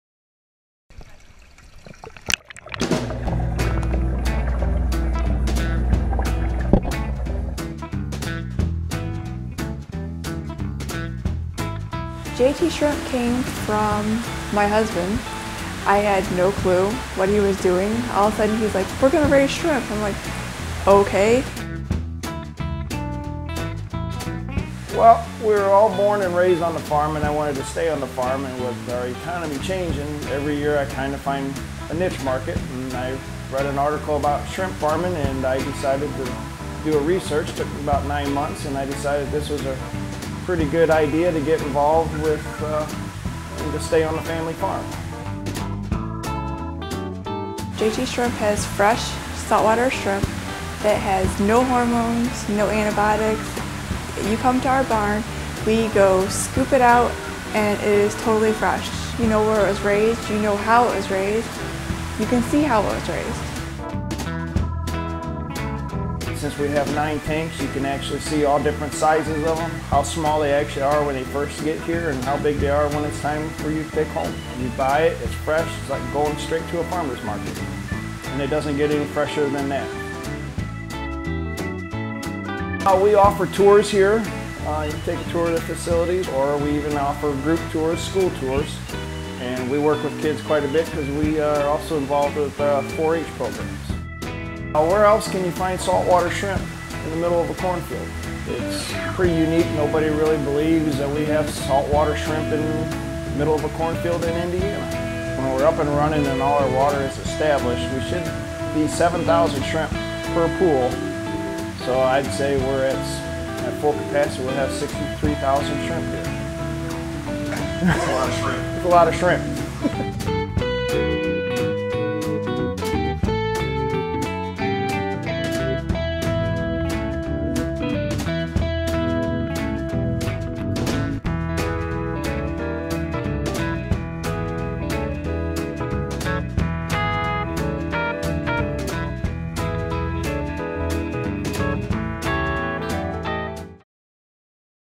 JT Shrimp
JT Shrimp brings something unique and refreshing to the South Shore, fresh salt water shrimp! Dive into the video and learn about the shrimp farm and it's tours!

JTShrimp.com
AlongTheSouthShore.com

Any rebroadcast, retransmission, use by third party or account of this video, without the express written consent and permission of the Lake County Convention and Visitors Bureau (dba South Shore Convention and Visitors Authority) is prohibited.